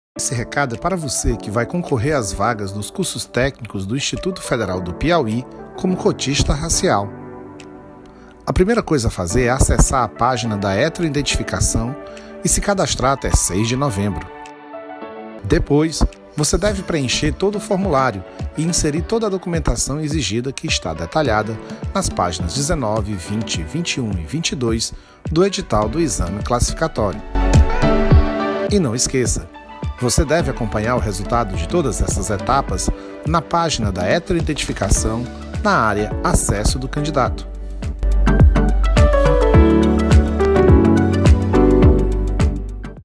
Processo de Heteroidentificação do Exame Classificatório 2023.1
Confira orientações para os candidatos que vão concorrer às vagas por cotas raciais do Exame Classificatório 2023.1 do Instituto Federal do Piauí.